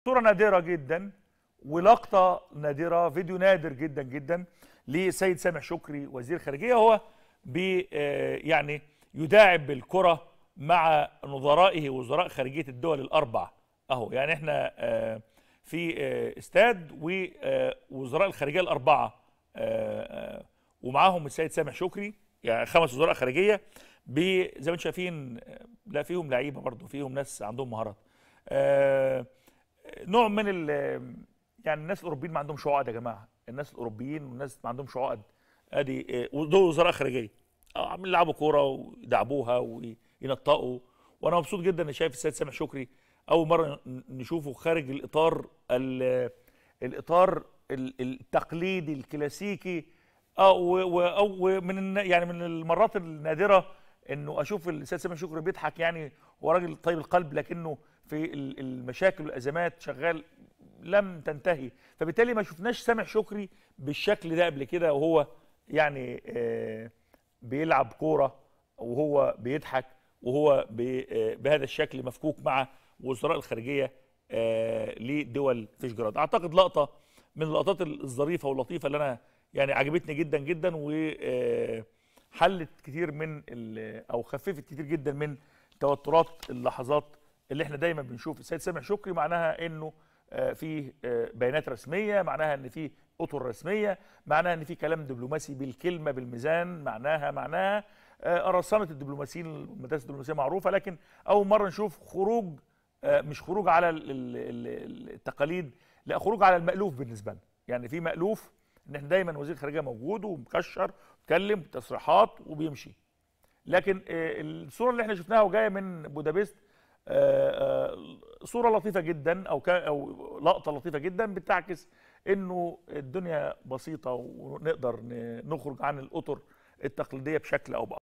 فيديو نادر جدا يظهر فيه وزير الخارجية سامح شكري مع وزراء خارجية دول فيشجراد يلعبون كرة القدم
• تابعوا الصفحة الرسمية للإعلامي نشأت الديهي على الفيسبوك ➭